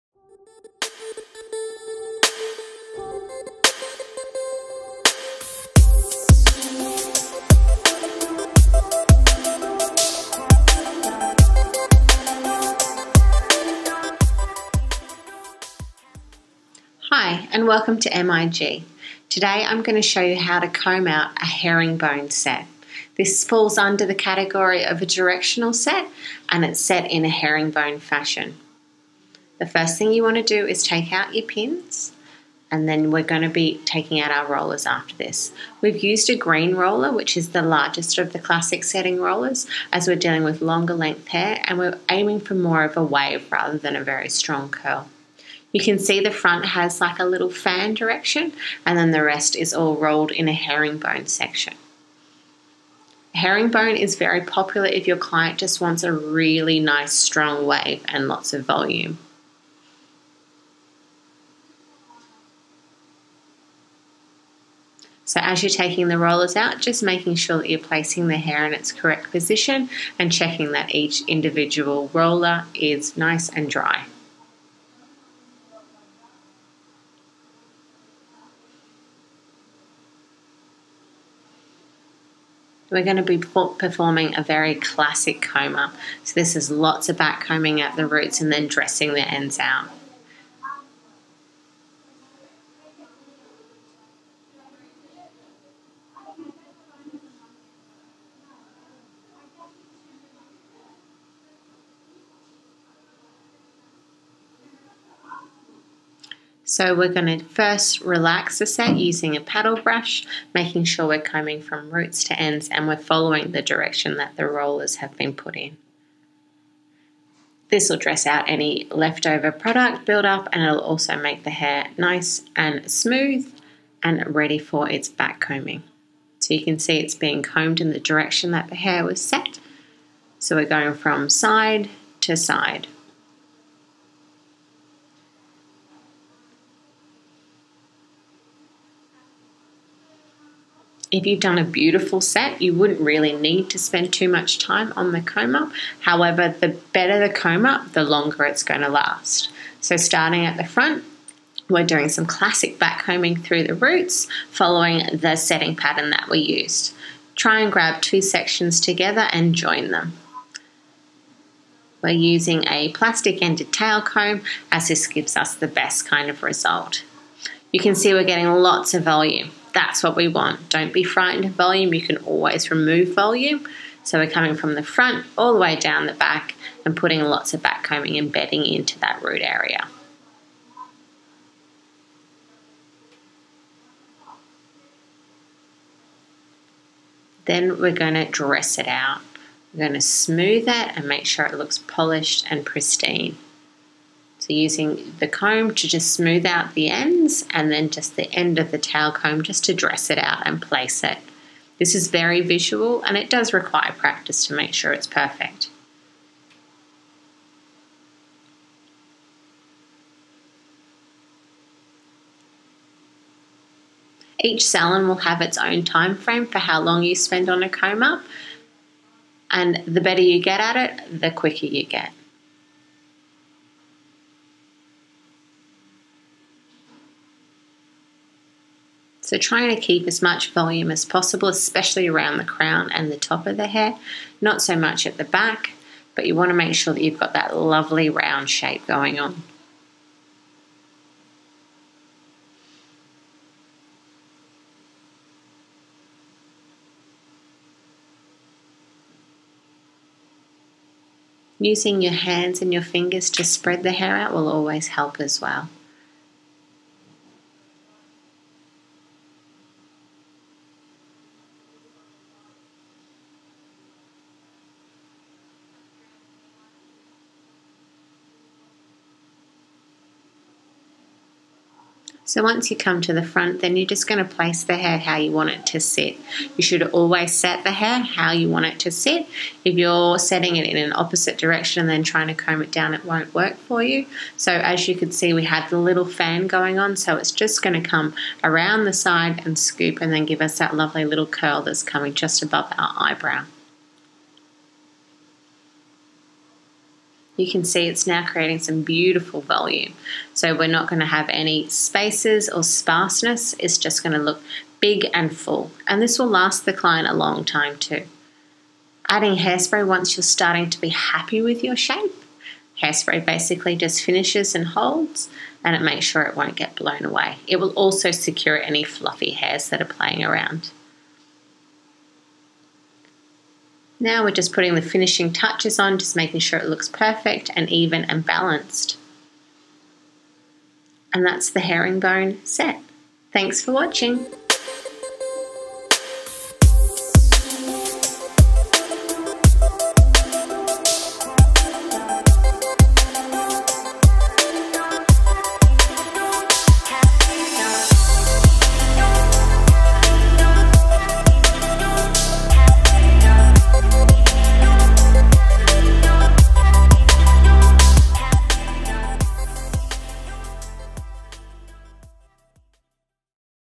How to comb up a set.
Classic techniques that help build volume and shape.

Filmed  in our beautiful training studio as a how to for the Hairdressing apprentices and students we train. Hope you find this easy to follow.